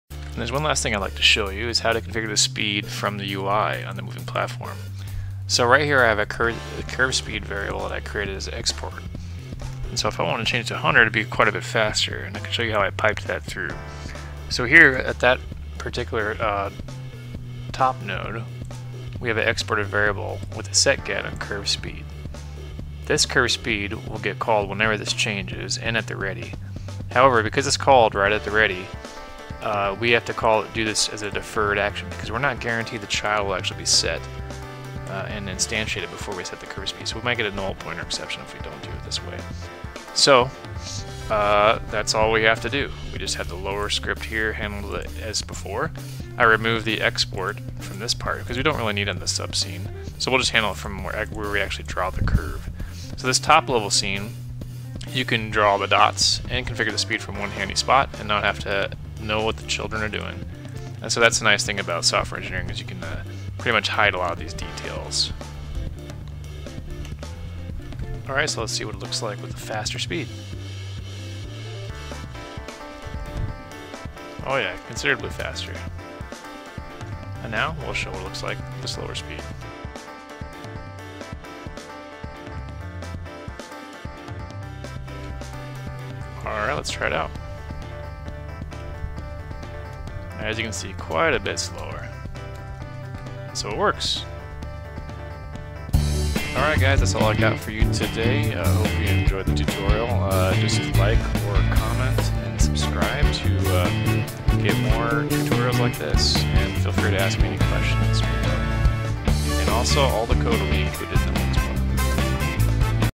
Godot Tutorial: Easy Speed Setting of One Way Collision Moving Platform
Hi Fellow GameDevs,

This is Tutorial we will learn how easily configure Speed for a One Way Collision Moving
platforms.

If you missed it, please watch checkout the first video on creating One Way

This is a supplement to the previous tutorial on One Way Collision Platforms.

What is one way collision?

It means you can jump from the bottom freely and pass through the platform
while landing on top.  Similar to what is used by Super Mario Brothers types
of games.

In this video I'll show how to:

- The code and config for piping platform speed through UI to the platform
- Learn how parent nodes can easily control their children through exported
  parameters
- Learn about using gdscript's setget

Tools used:
- Godot: 3.1.1